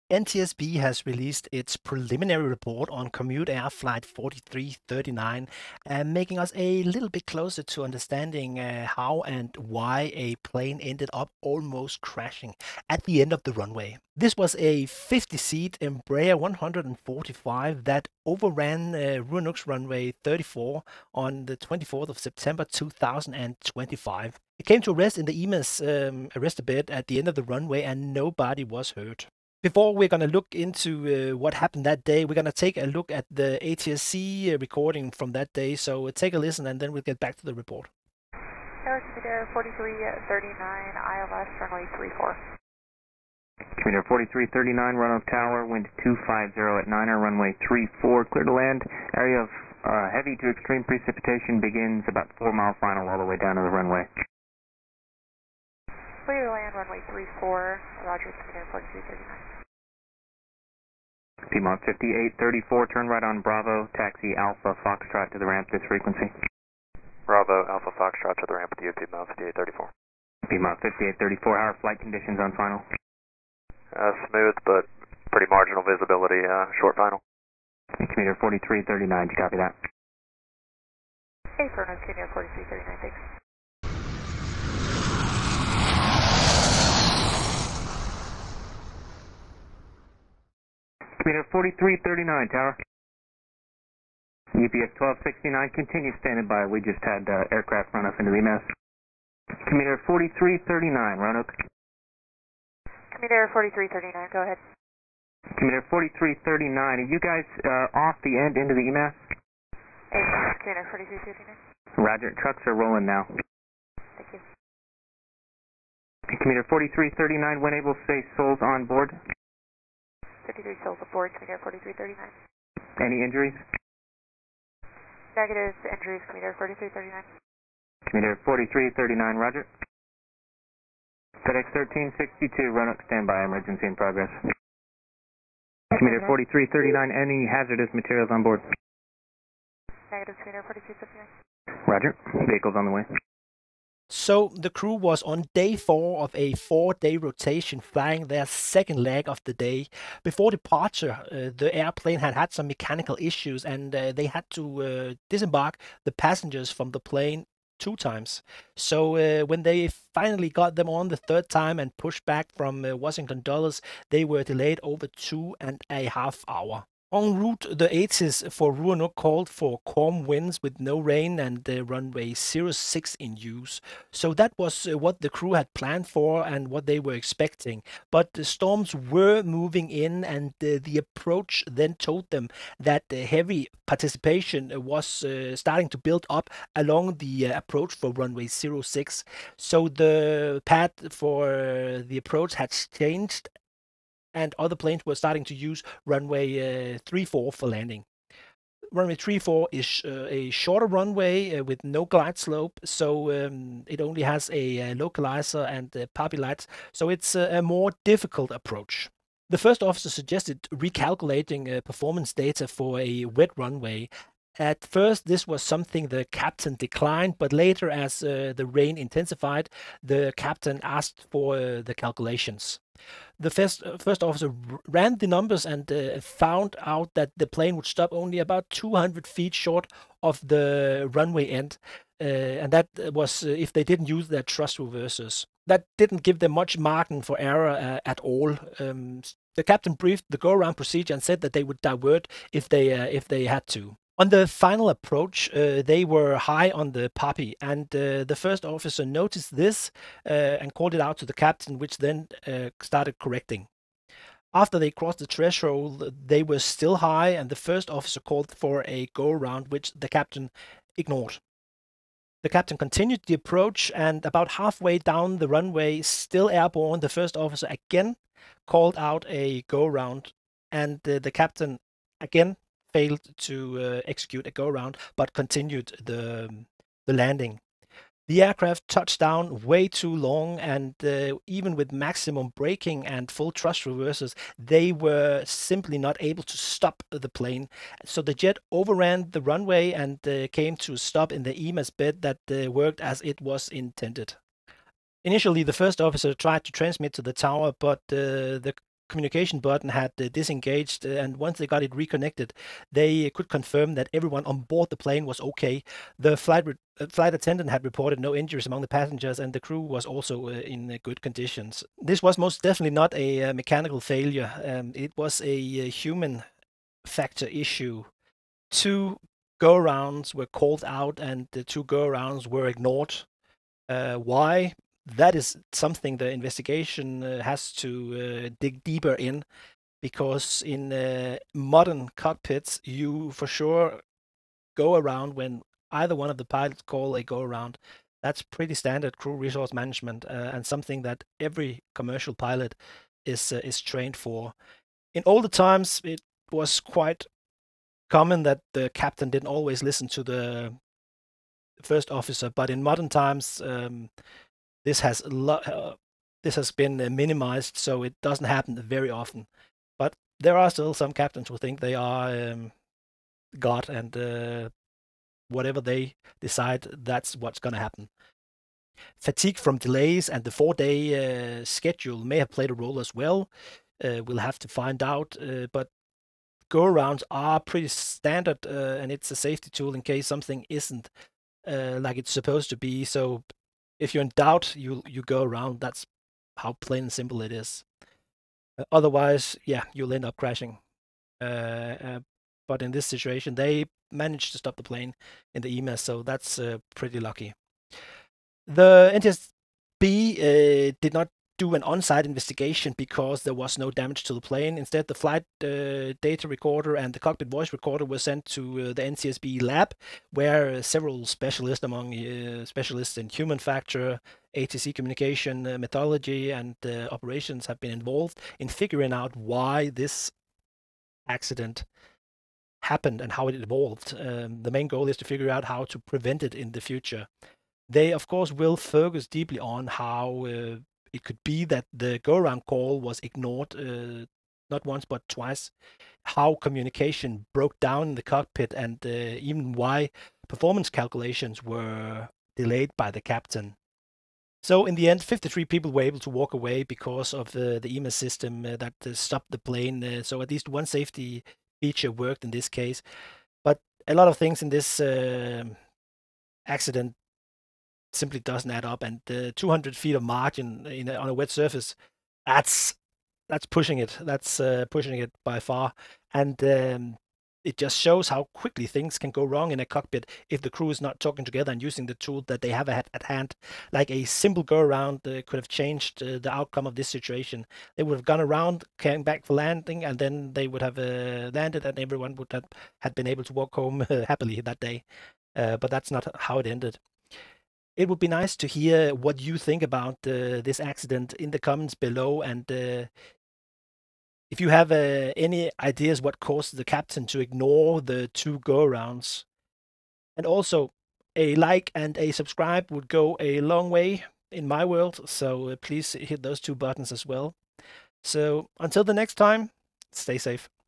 So Many Warnings. One Captain. Multiple Terrible Decisions.
On the night of September 24th, 2025, CommuteAir Flight 4339/United Express 4339 — a 50-seat Embraer ERJ-145XR — overran Runway 34 at Roanoke-Blacksburg Regional Airport. It was raining. The runway was wet. And the margin for error was razor-thin.

This video takes you through the timeline of the incident, the weather conditions, and — most importantly — the decisions made in the cockpit. The crew had originally planned to land on the longer Runway 6, but were rerouted last-minute to Runway 34. It’s shorter, lacks vertical guidance, and on this night… it was slick.

The first officer suggested recalculating landing performance for a wet runway. At first, the captain declined. Later, they ran the numbers: with no thrust reversers accounted for, the aircraft would have just 200 feet of stopping distance remaining — assuming a perfect touchdown. But the approach was high. The FO called for a go-around. Twice. But the aircraft continued. And by the time it touched down — long and fast — there simply wasn’t enough runway left.

Fortunately, Roanoke had installed an EMAS (Engineered Materials Arresting System) bed just a year earlier. That system — a crushable surface designed to safely stop aircraft in runway overruns — did its job perfectly. The jet came to a full stop inside the EMAS. Everyone walked away.

In this video, we’ll break down:

The weather, runway change, and flight crew performance.

Why go-arounds are a vital safety tool.

How human factors like fatigue and delay might have influenced this decision.

And how EMAS systems are quietly saving lives at airports around the world.

We’ll also look at what the NTSB’s preliminary report reveals, and what investigators are likely focusing on next.

🎧 Includes ATC audio - credit to LiveATC
📊 Includes technical breakdown
🛫 Real-world aviation lessons

Do you think the go-around should have been mandatory the moment the margin was calculated?

00:38 – Live ATC recording
02:20 – Flight background and maintenance issues
02:45 – Runway change and weather deteriorates
04:00 – Go-around ignored: twice
05:00 – Human factors and fatigue
06:43 – NTSB investigation and focus areas